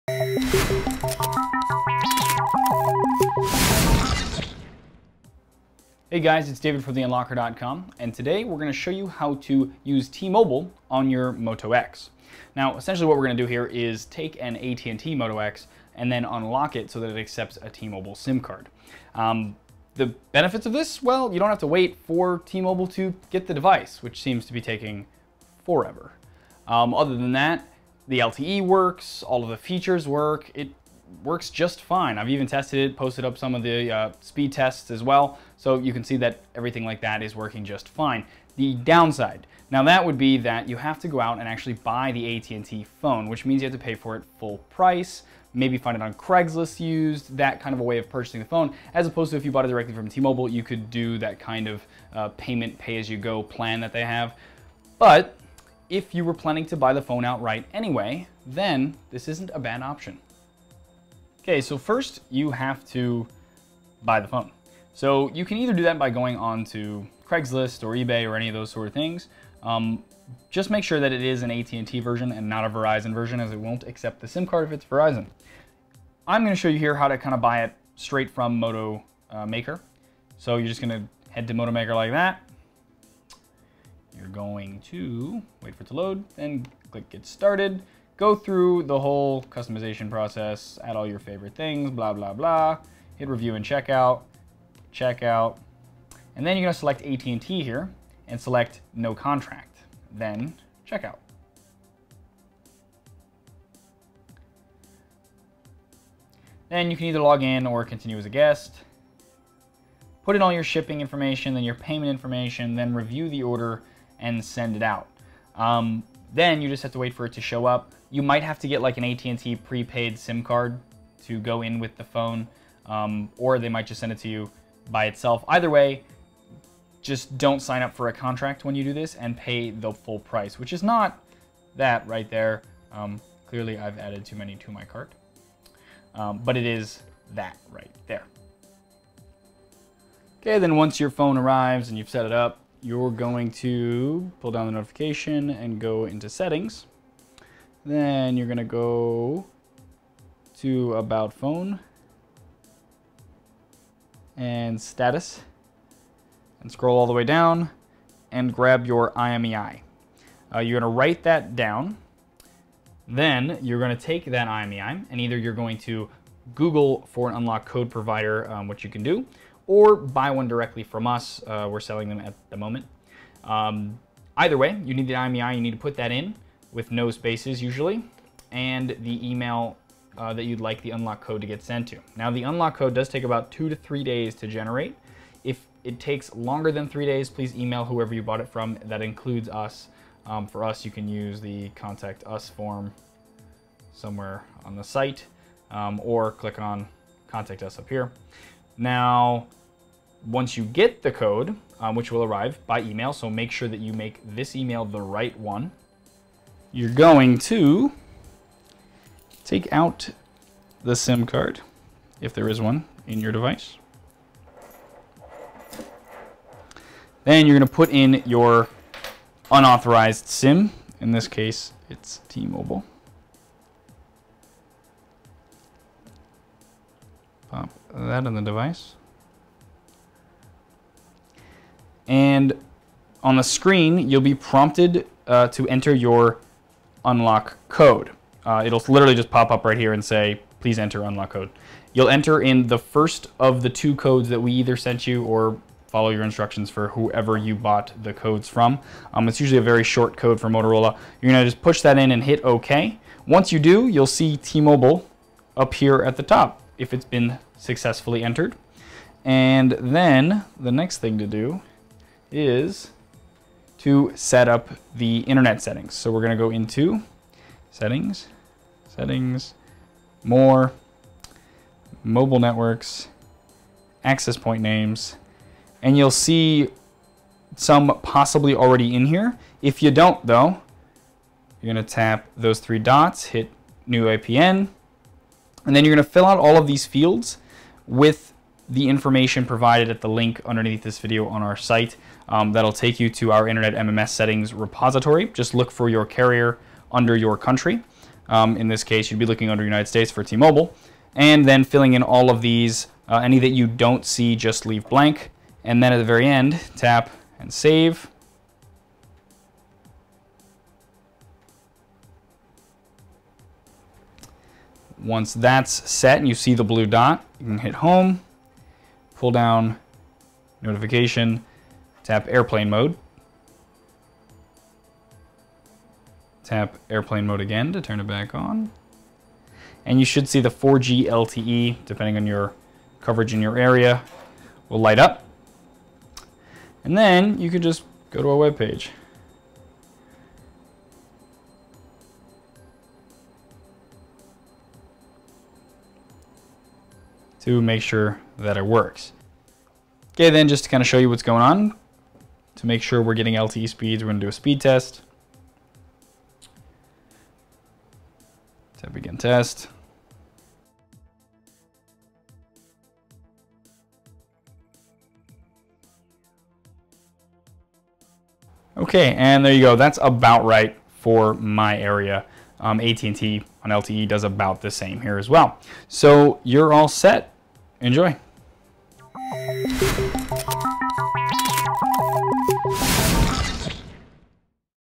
How to Use TMobile on the Moto X Right Now
Head to our site above for the necessary files or to comment and to see our other videos.

Sick of waiting for the T-Mobile Moto X to launch? Well, if you were planning to buy it without a contract anyway, there is a way to get it right now. All you have to do is buy an AT&amp;T version (new from Motorola directly without contract, or used via craigslist/ebay/etc.) and unlock it. The unlock is permanent not only will allow you to put in a T-Mobile SIM but any other SIM card for that matter (say overseas, SimpleMobile, or any other GSM provider). Interested? Here's how.